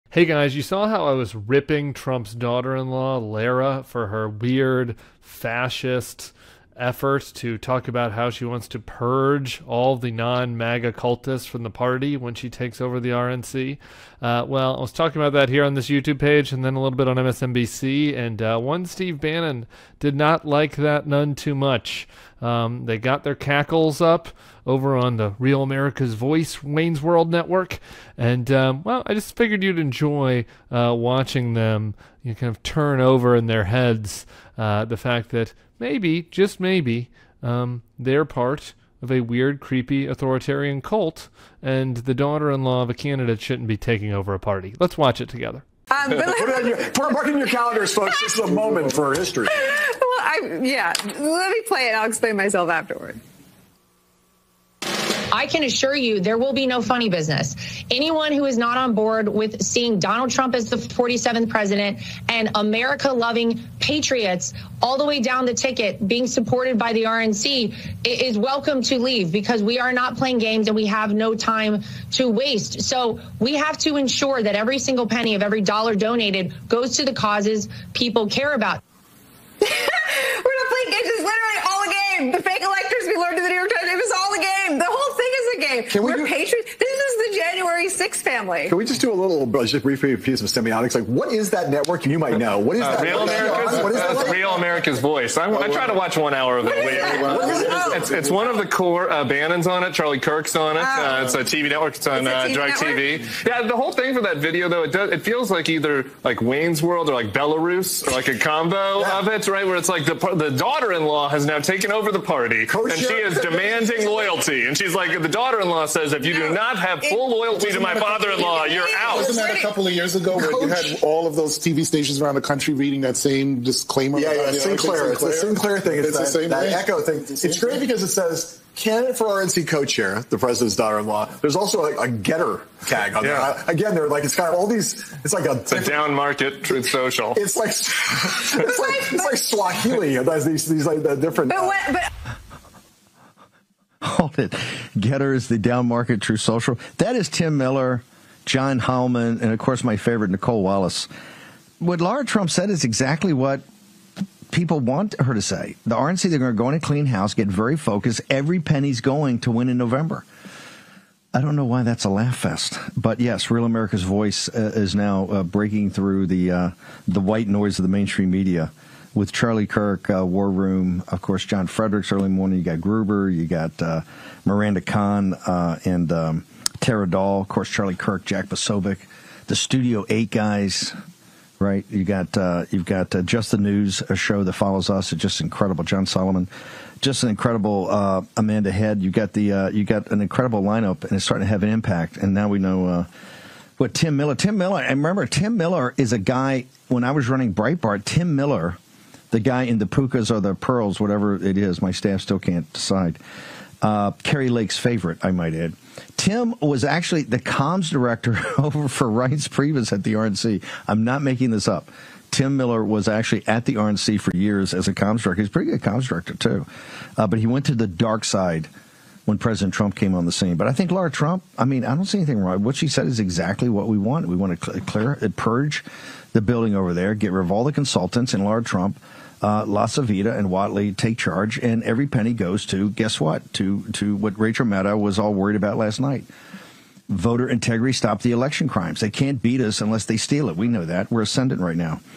Steve Bannon is TRIGGERED by Tim Miller Calling Out the Trump Cult! | Clip
Tim Miller went on MSNBC and chastised Lara Trump for demanding loyalty to Donald Trump, and Steve Bannon did not take it well!